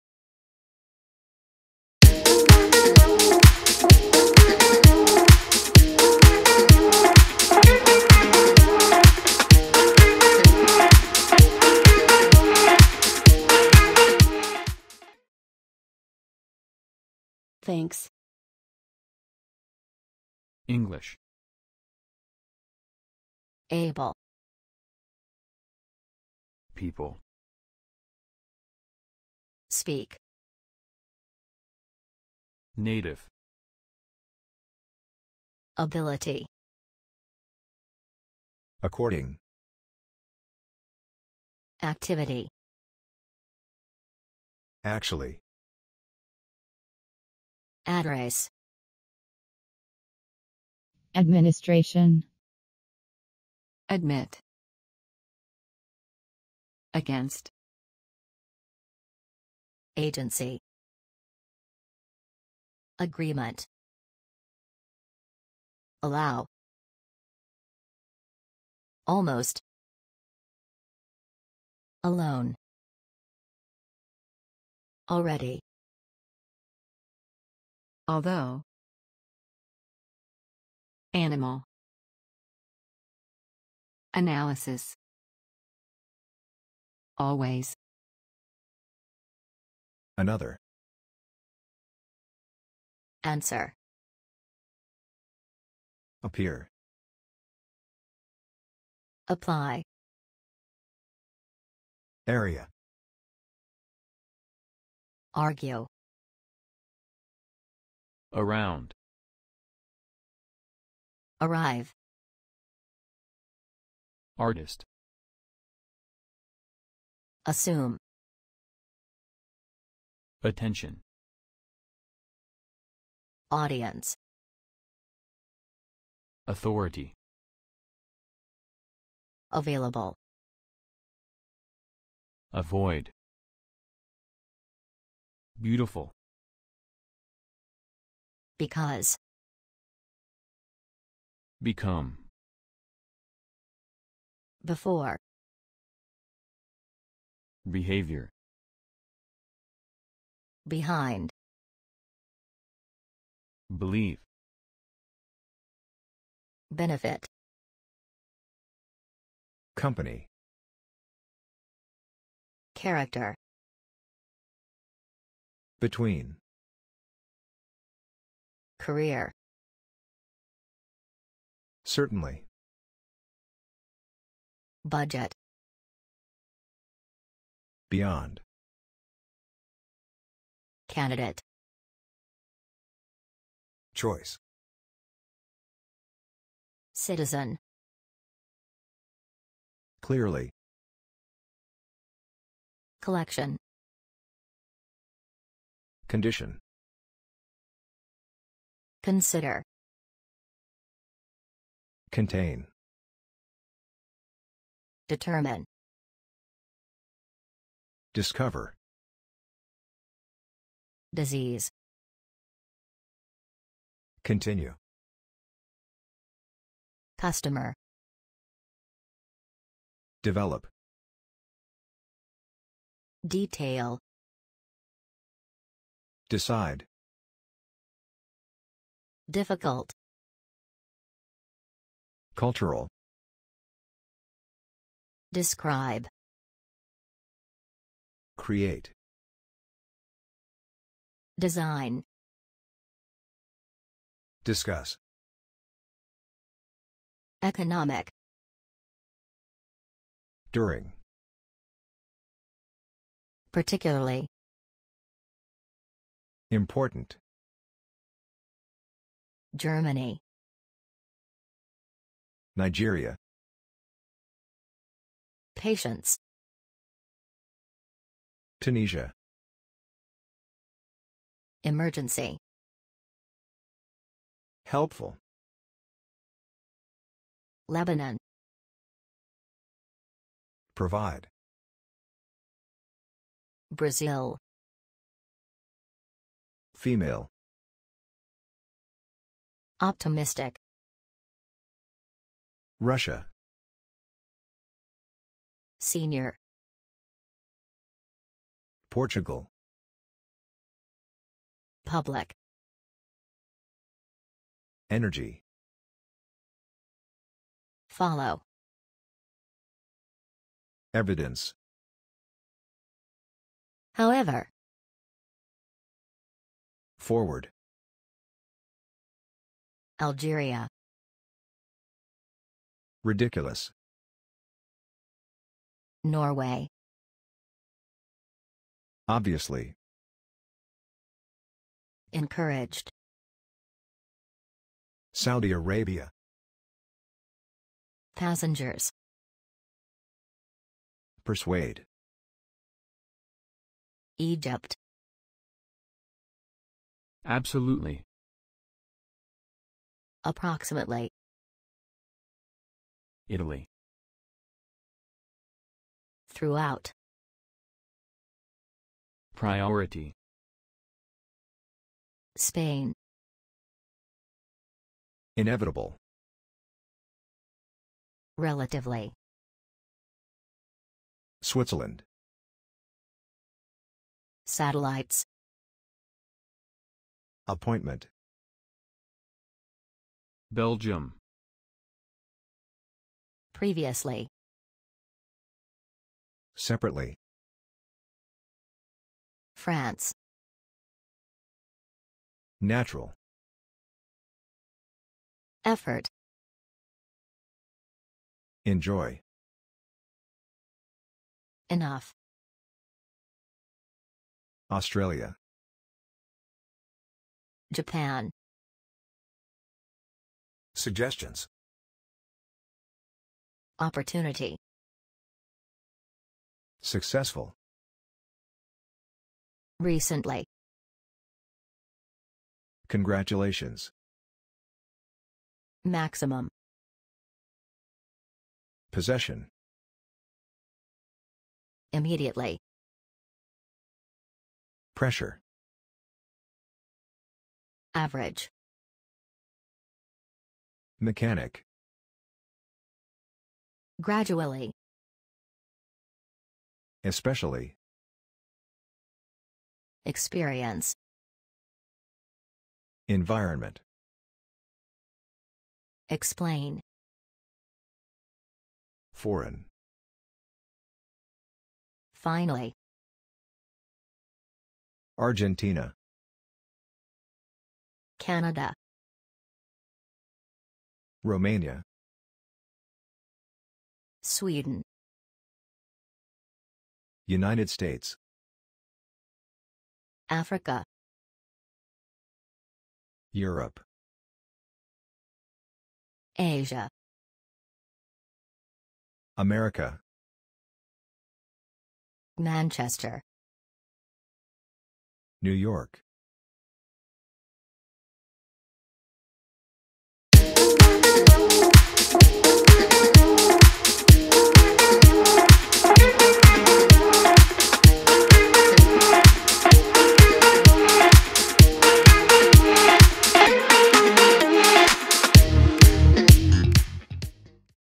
PRONUNCIATION OF BASIC WORDS OF ENGLISH 1.mp4
Pononciation de plus de 150 mots de base en anglais.